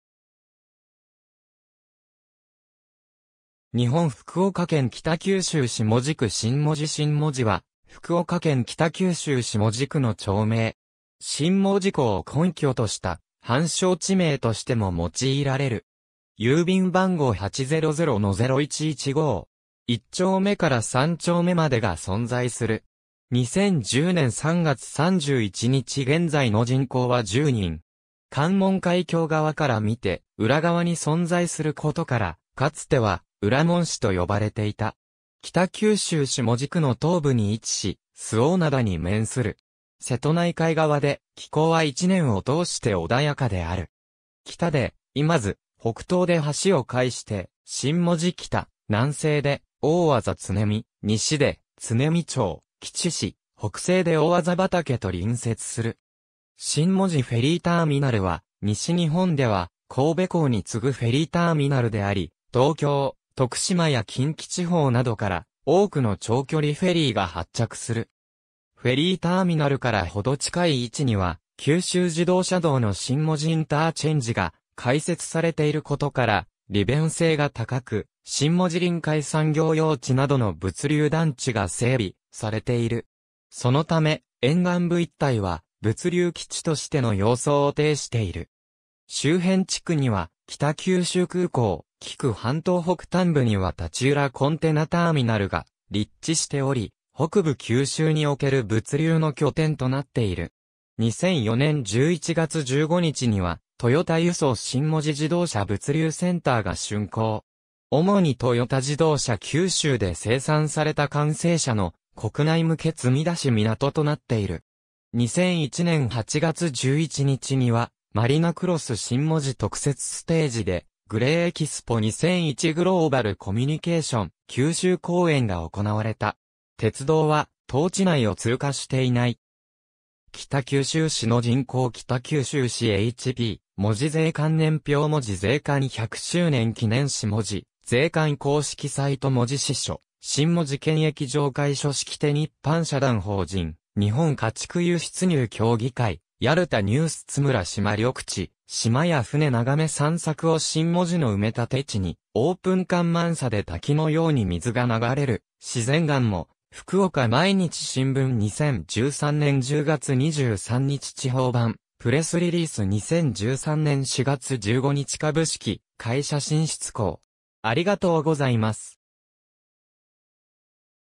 新門司
北九州市の町・字
日本 」 福岡県 」 北九州市 」 門司区 」 新門司 新門司（しんもじ）は福岡県北九州市門司区の町名。
新門司港（北九州港）を根拠とした汎称地名としても用いられる。
1丁目から3丁目までが存在する。
2010年3月31日現在の人口は10人。
関門海峡側からみて裏側に存在することから、かつては裏門司（うらもじ）と呼ばれていた。
北九州市門司区の東部に位置し、周防灘に面する。
瀬戸内海側で気候は1年を通して穏やかである。
北で今津、北東で橋を介して新門司北、南西で大字恒見、西で恒見町・吉志、北西で大字畑と隣接する。
新門司フェリーターミナルは西日本では神戸港に次ぐフェリーターミナルであり、東京、徳島や近畿地方などから多くの長距離フェリーが発着する。
フェリーターミナルから程近い位置には九州自動車道の新門司インターチェンジが開設されている事から、利便性が高く、新門司臨海産業用地（マリナクロス新門司）などの物流団地が整備されている。
そのため、沿岸部一帯は物流基地としての様相を呈している。
周辺地区には北九州空港、企救半島北端部には太刀浦コンテナターミナルが立地しており、北部九州における物流の拠点となっている。
2004年11月15日には、トヨタ輸送新門司自動車物流センターが竣工。
主にトヨタ自動車九州で生産された完成車の国内向け積み出し港となっている。
2001年8月11日にはマリナクロス新門司特設ステージでGLAY EXPO 2001 "GLOBAL COMMUNICATION"九州公演が行なわれた。
鉄道は当地内を通過していない。
^ 北九州市の人口（町別）-北九州市HP ^ 門司税関年表 門司税関100周年記念誌門司税関公式サイト ^ 門司支所 新門司検疫場　開所式典一般社団法人日本家畜輸出入協議会 JLTAニュース ^ 津村島緑地:島や船眺め散策を 新門司の埋め立て地にオープン 干満差で滝のように水が流れる自然岩も/福岡毎日新聞2013年10月23日地方版 ^ プレスリリース2013年4月15日 株式会社新出光